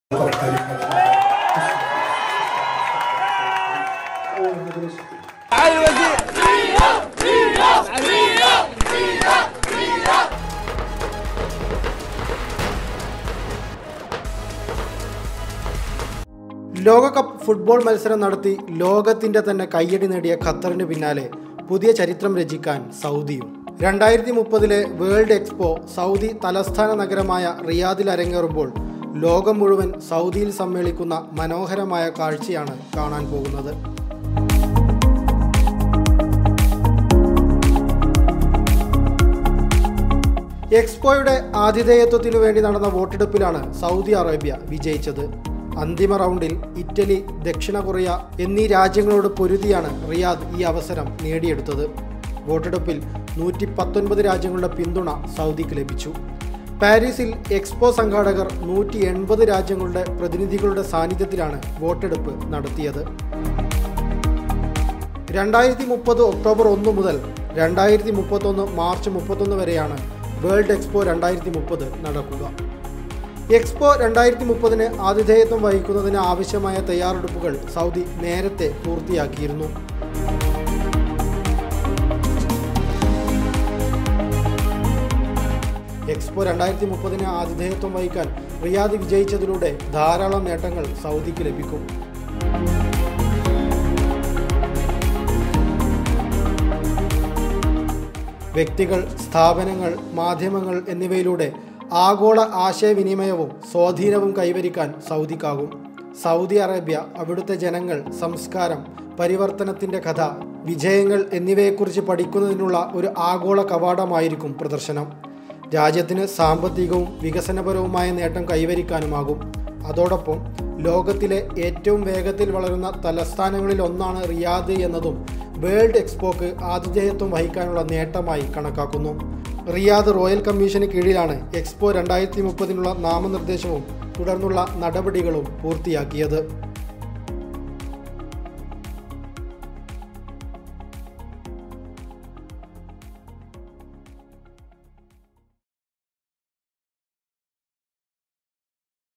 2030ൽ ലോകം റിയാദിൽ; ആഗോള കവാടമാവാനൊരുങ്ങി സൗദി | Riyadh Expo 2030
ഖത്തറിന് പിന്നാലെ പുതിയ ചരിത്രം രചിക്കാൻ സൗദിയും.

 Riyadh Expo 2030